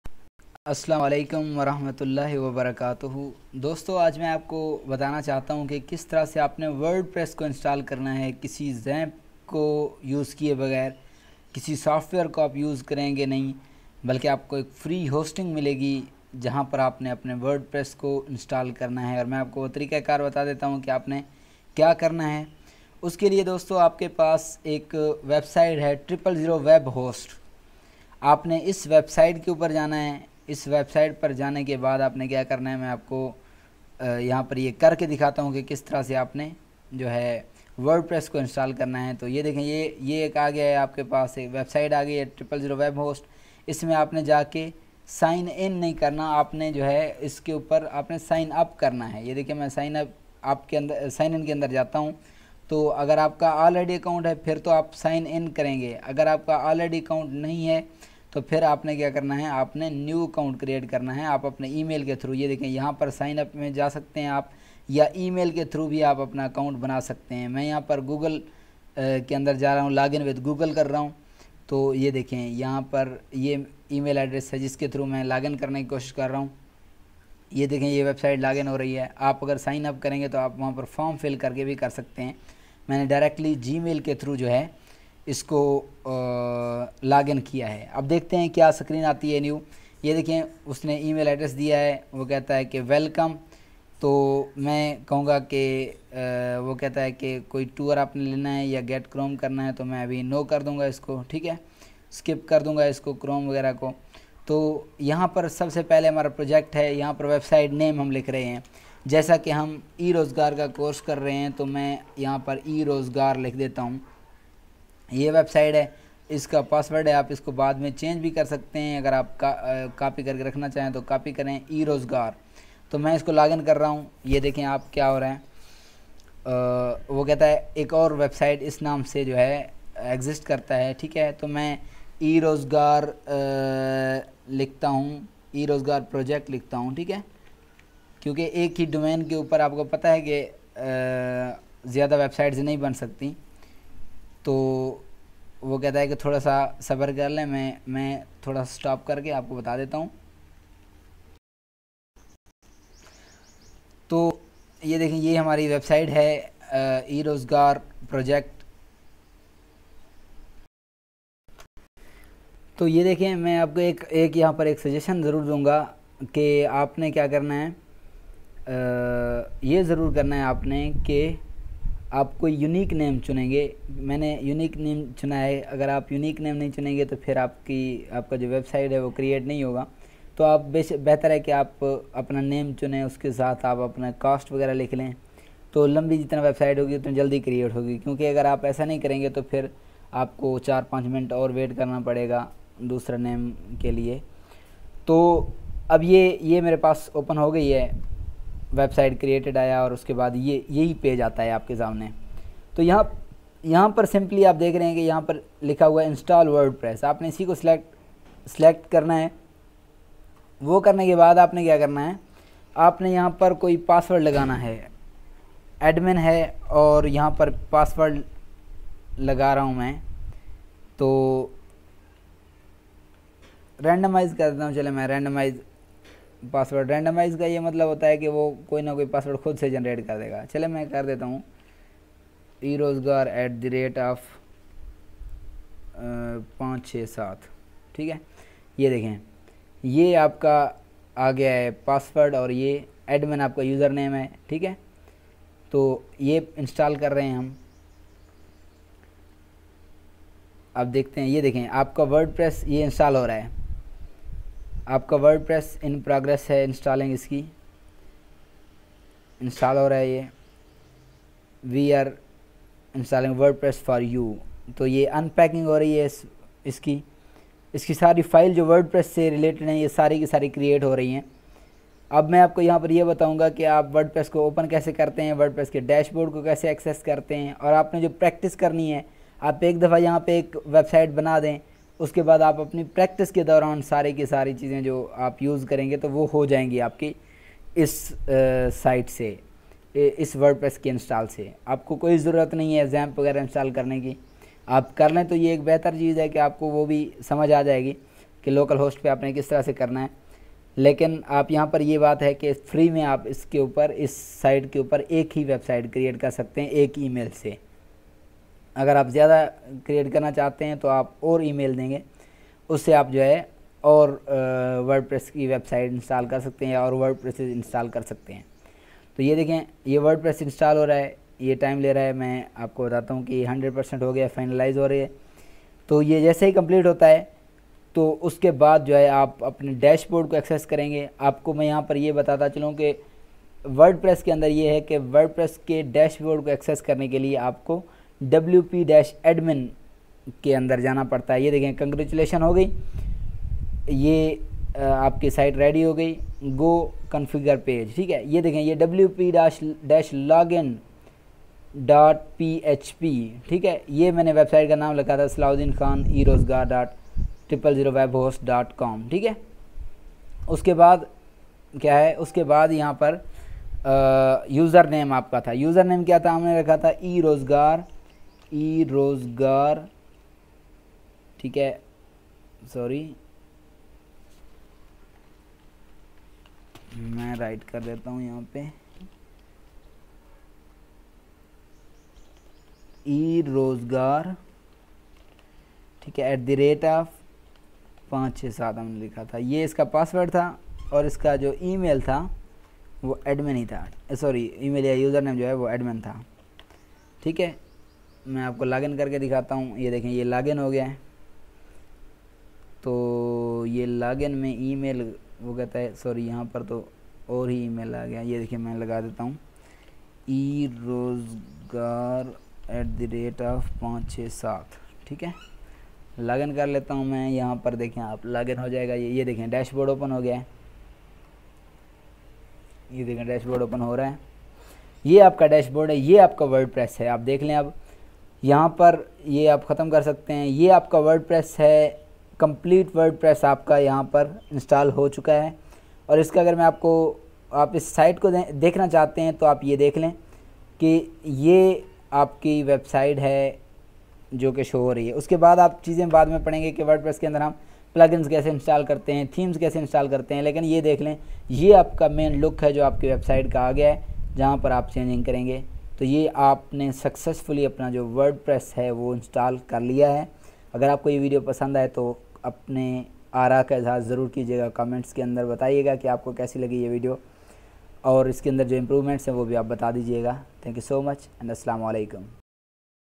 Install WordPress Online on Free Web hosting 000webhost.com [without Xampp] || Urdu/Hindi Tutorial
In this video we will learn how to install WordPress online  on free webhost without using xampp.
There is a website 000webhost.com from where we can install wordpress for free.